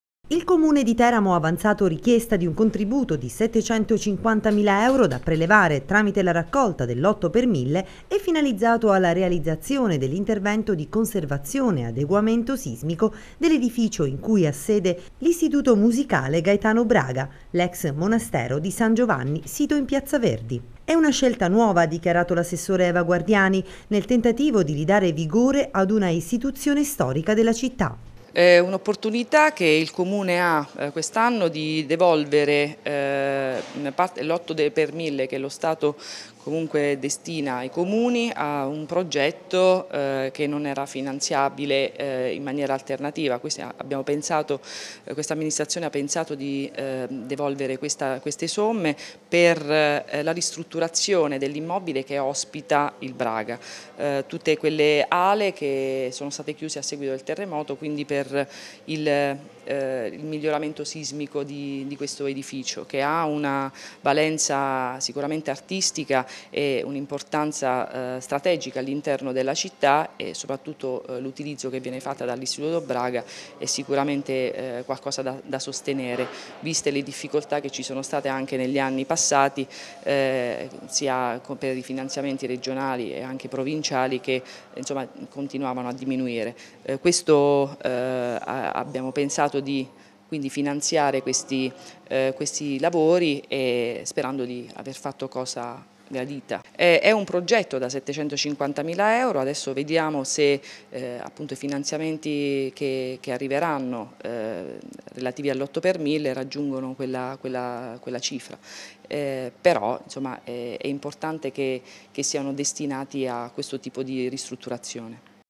Teramo 8x1000 per il Braga
Il Comune di Teramo ha avanzato richiesta agli Uffici della Presidenza del Consiglio dei Ministri, per l'attivazione della donazione de 8X1000 a favore dell'Istituto Superiore di Studi Musicali Gaetano Braga